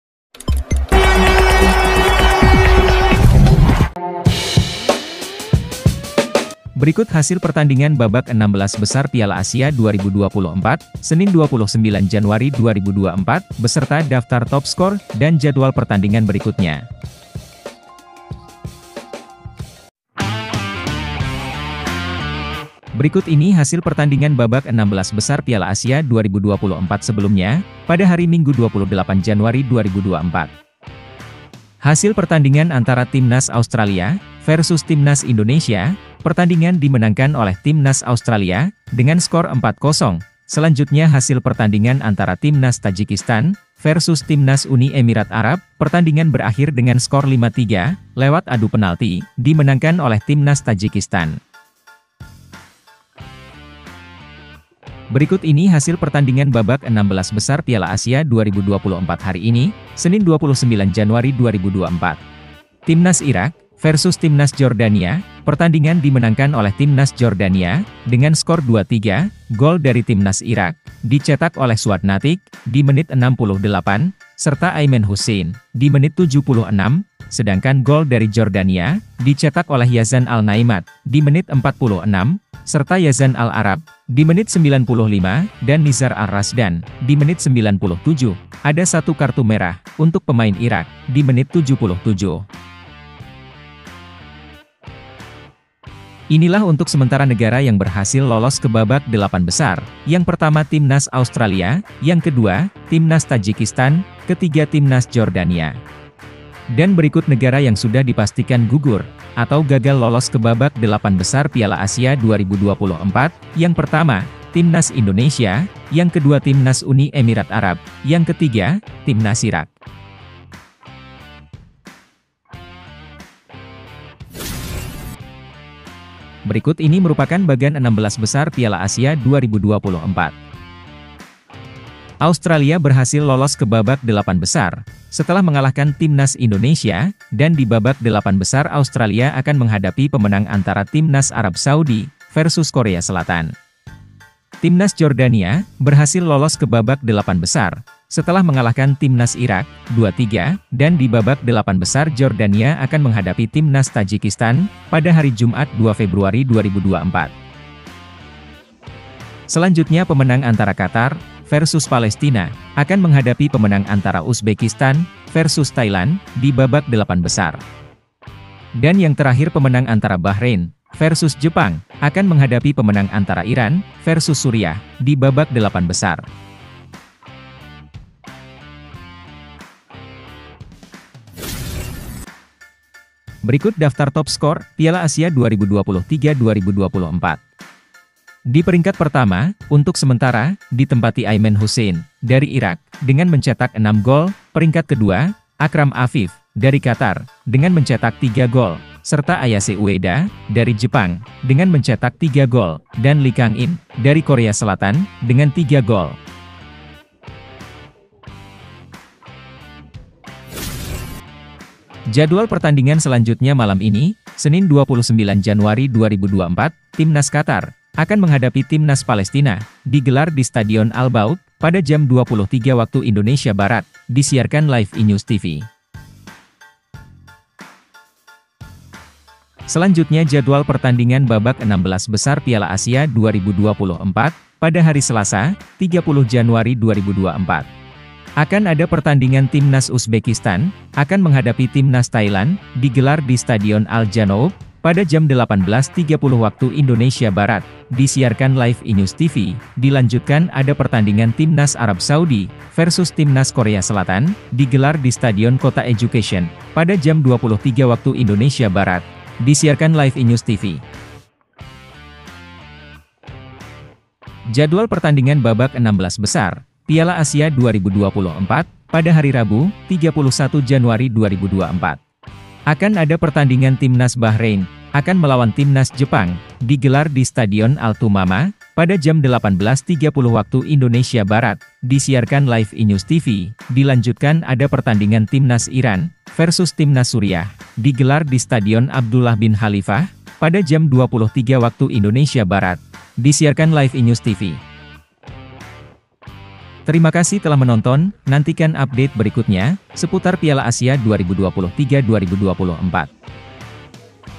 Hasil Piala Asia 2024 - Irak vs Jordania - Bagan 16 Besar Piala Asia Qatar 2023 Terbaru
Hasil Piala Asia 2024 - Irak vs Jordania - Bagan 16 Besar Piala Asia Qatar 2023 Terbaru - AFC ASIAN CUP 2023
Berikut hasil piala asia hari ini :
senin 29  januari 2024
irak 2-3 jordania